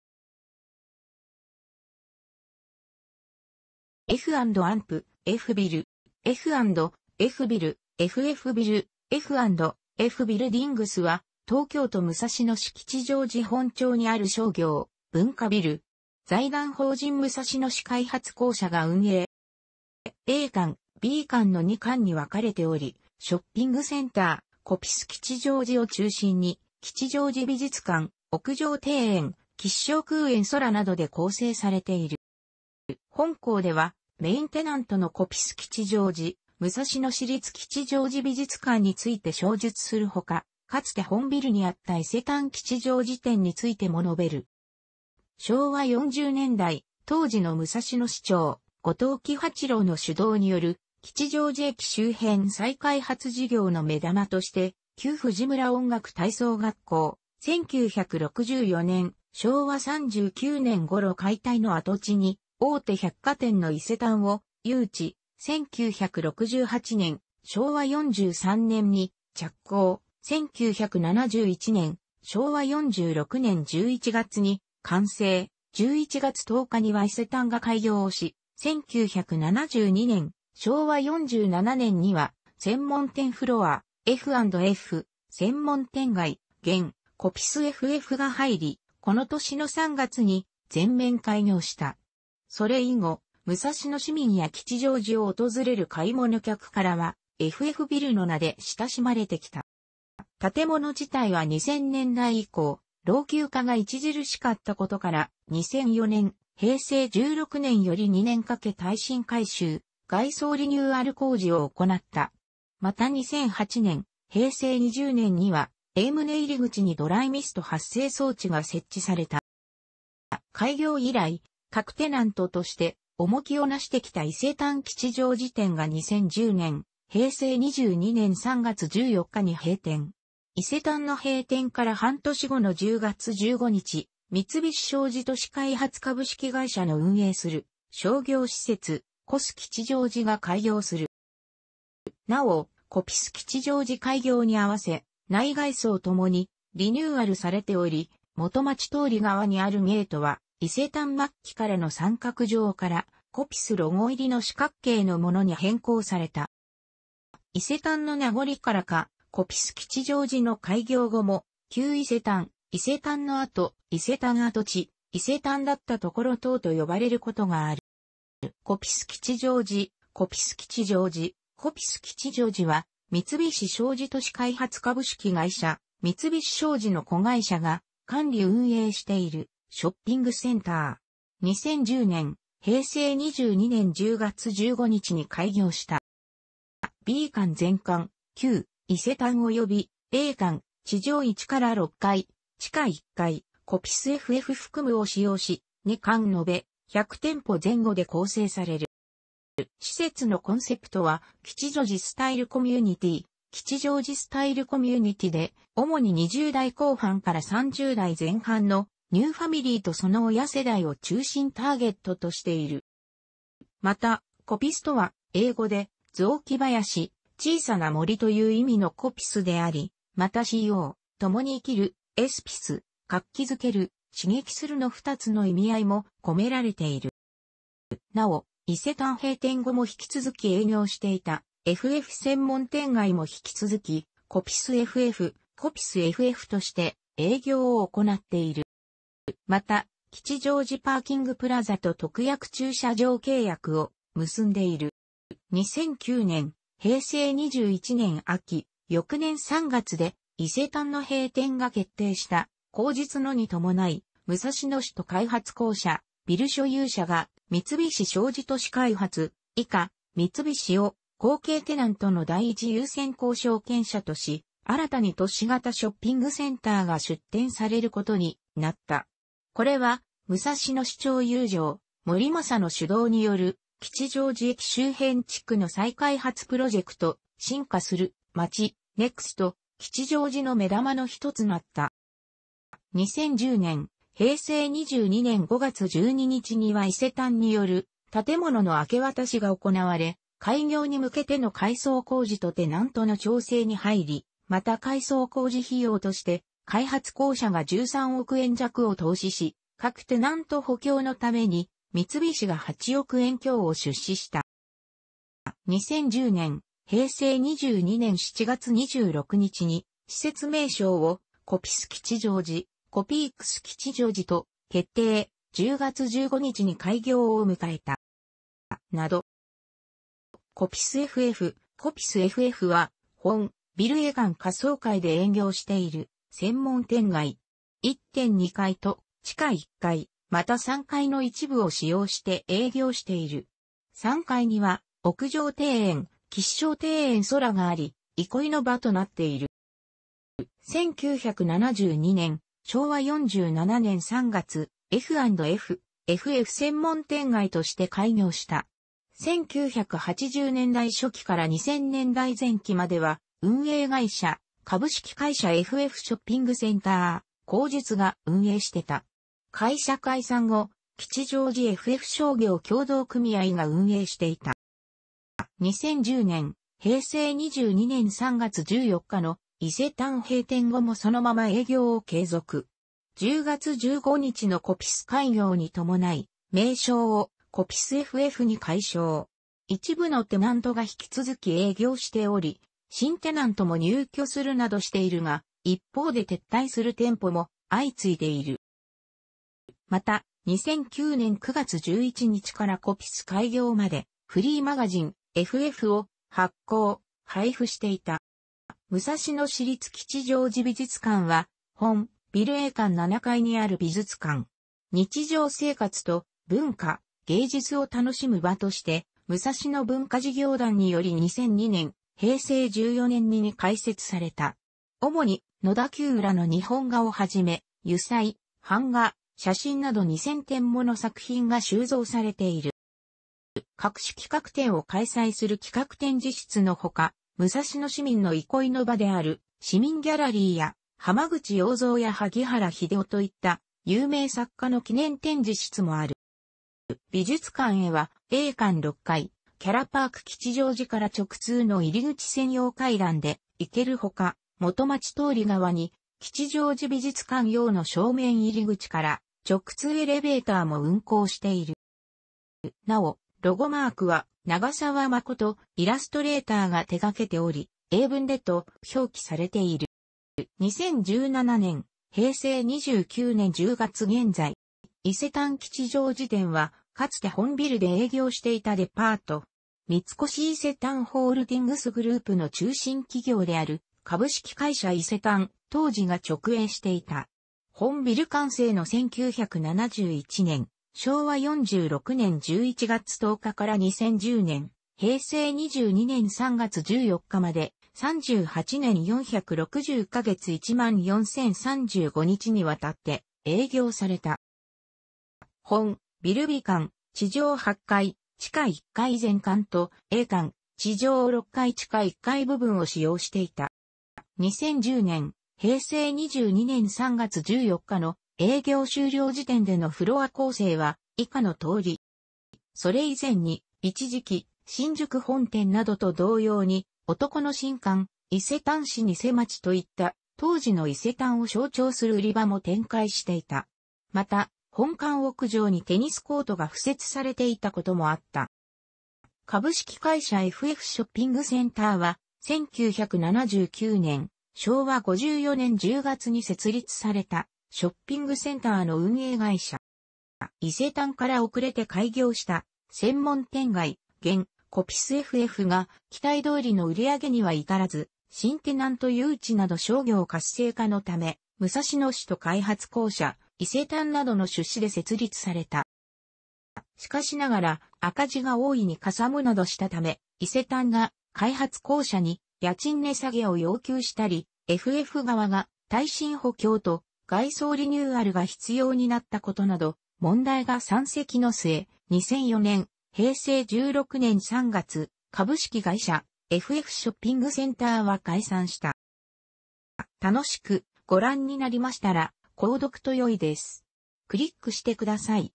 F&amp;Fビル
F&Fビル（エフエフビル、"F&F Buildings"）は、東京都武蔵野市吉祥寺本町にある商業・文化ビル。財団法人武蔵野市開発公社が運営。

A館、B館の2館に分かれており、ショッピングセンター『コピス吉祥寺』を中心に、吉祥寺美術館、屋上庭園「吉祥空園sora」などで構成されている。

本項ではメインテナントのコピス吉祥寺、武蔵野市立吉祥寺美術館について詳述するほか、かつて本ビルにあった伊勢丹吉祥寺店についても述べる。

昭和40年代、当時の武蔵野市長・後藤喜八郎の主導による吉祥寺駅周辺再開発事業の目玉として、旧藤村音楽体操学校（1964年（昭和39年）ごろ解体）の跡地に大手百貨店の伊勢丹を誘致、1968年（昭和43年）に着工、1971年（昭和46年）11月に完成（11月10日には伊勢丹が開業）し、1972年（昭和47年）には専門店フロア『F&F・専門店街』（現・コピスエフエフ）が入り、この年の3月に全面開業した。それ以後、武蔵野市民や吉祥寺を訪れる買い物客からは「FFビル」の名で親しまれてきた。

建物自体は2000年代以降、老朽化が著しかったことから、2004年（平成16年）より2年かけ耐震改修・外装リニューアル工事を行った。また2008年（平成20年）にはA棟入口にドライミスト発生装置が設置された。

開業以来、核テナントとして重きをなしてきた伊勢丹吉祥寺店が2010年（平成22年）3月14日に閉店。伊勢丹の閉店から半年後の10月15日、三菱商事都市開発株式会社の運営する商業施設「コピス吉祥寺」が開業する。

なお「コピス吉祥寺」開業に合わせ、内外装ともにリニューアルされており、元町通り側にあるゲートは、伊勢丹末期からの三角状からCoppiceロゴ入りの四角形のものに変更された。

伊勢丹の名残からか、コピス吉祥寺の開業後も「旧伊勢丹」「伊勢丹の跡」「伊勢丹跡地」「伊勢丹だった所」等と呼ばれることがある。

コピス吉祥寺（コピスきちじょうじ、coppice KICHIJOJI）は、三菱商事都市開発株式会社（三菱商事の子会社）が管理運営しているショッピングセンター。2010年（平成22年）10月15日に開業した。

B館全館（旧・伊勢丹）および A館地上1 - 6階・地下1階（コピスエフエフ含む）を使用し、2館述べ、100店舗前後で構成される。

施設のコンセプトは「Kichijoji Style Community（吉祥寺スタイル・コミュニティ）」で、主に20代後半 - 30代前半のニューファミリーとその親世代を中心ターゲットとしている。

また「コピス」とは、英語で「雑木林・小さな森」という意味の"Coppice"であり、またco（共に生きる）+(s)pice（活気づける・刺激する）の2つの意味合いも込められている。

なお、伊勢丹閉店後も引き続き営業していたエフエフ専門店街も引き続き、コピスエフエフ (coppice ff) として営業を行っている。

また、吉祥寺パーキングプラザと特約駐車場契約を結んでいる。

2009年（平成21年）秋、翌年3月で伊勢丹の閉店が決定した（後述）のに伴い、武蔵野市と開発公社（ビル所有者）が三菱商事都市開発（以下、三菱）を後継テナントの第一優先交渉権者とし、新たに都市型ショッピングセンターが出店されることになった。これは武蔵野市長邑上守正の主導による吉祥寺駅周辺地区の再開発プロジェクト『進化するまち「NEXT―吉祥寺」』の目玉の一つとなった。

2010年（平成22年）5月12日には伊勢丹による建物の明け渡しが行われ、開業に向けての改装工事とテナントの調整に入り、また改装工事費用として開発公社が13億円弱を投資し、各テナント補強のために三菱が8億円強を出資した。

2010年（平成22年）7月26日に、施設名称を『コピス吉祥寺』（"Copicce Kichijoji"）と決定、10月15日に開業を迎えた。

など

コピスエフエフ（coppice ff）は、本ビルA館下層階で営業している専門店街。1・2階と地下1階、また3階の一部を使用して営業している。3階には屋上庭園『吉祥庭園Sora』があり、憩いの場となっている。

1972年（昭和47年）3月、『F&F（エフ・エフ）専門店街』として開業した。1980年代初期から2000年代前期までは、運営会社「株式会社エフエフショッピングセンター」（後述）が運営していた。会社解散後、吉祥寺エフエフ商業協同組合が運営していた。

2010年（平成22年）3...